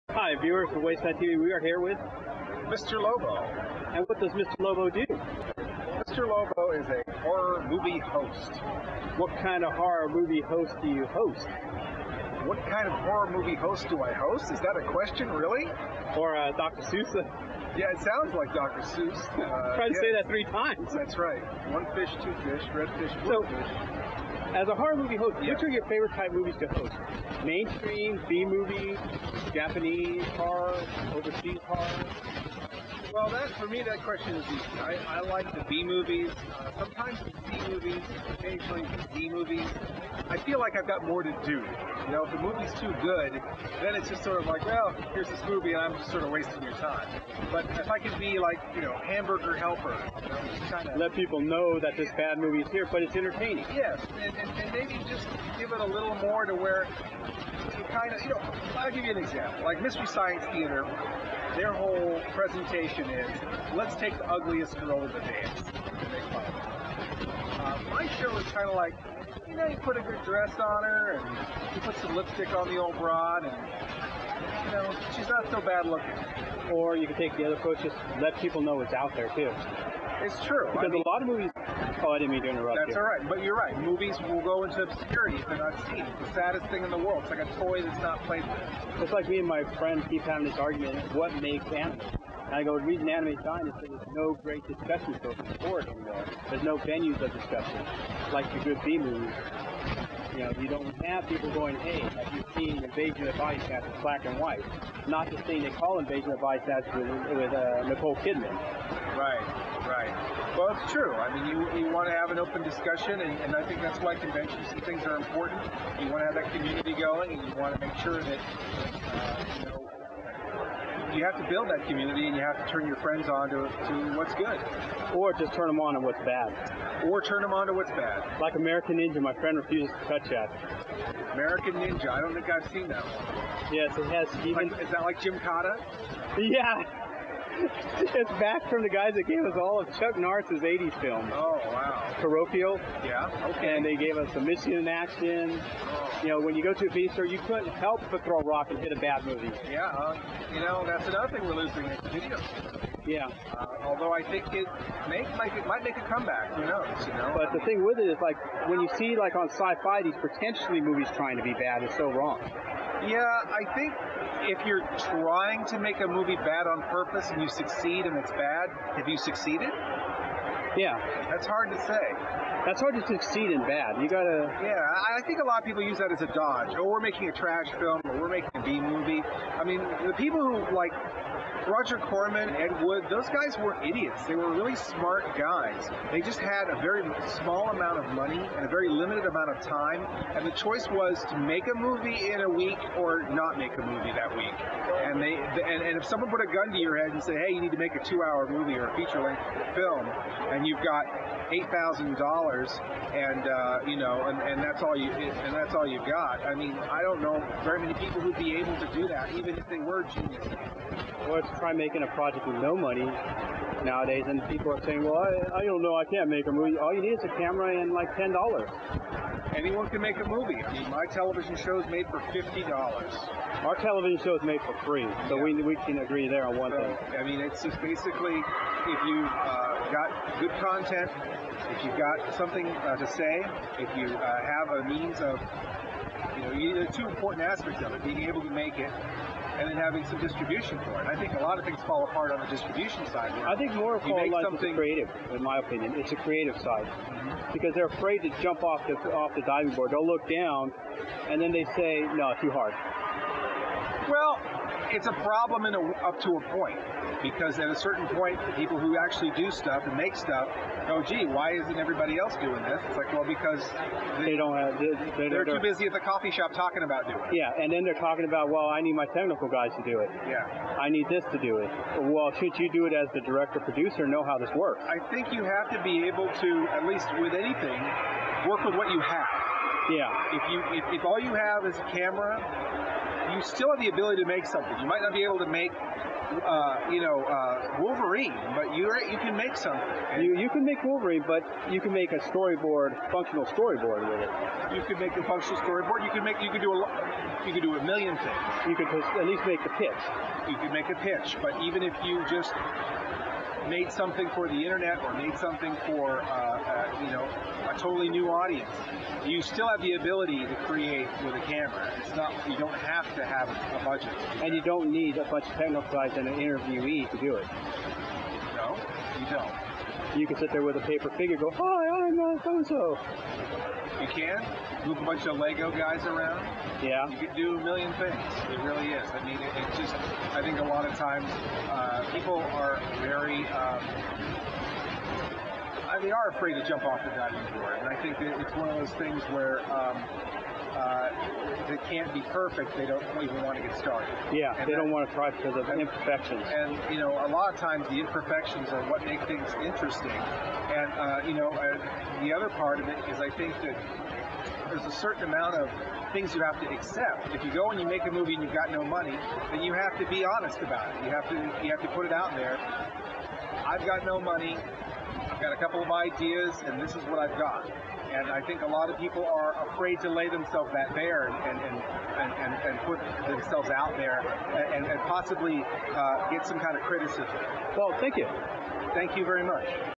Supercon 2009: Interview with Mr. Lobo
Agis261 interviews Mr. Lobo, who is a TV Horror Host from Sacramento. His TV show is called Cinema Insomnia, which can also be bought on DVD. He show such movies as Night of the Living Dead, The Terror, Gigantis, and the Prince of Space and remember in the words of Mr. Lobo, "They're not bad movies--just misunderstood".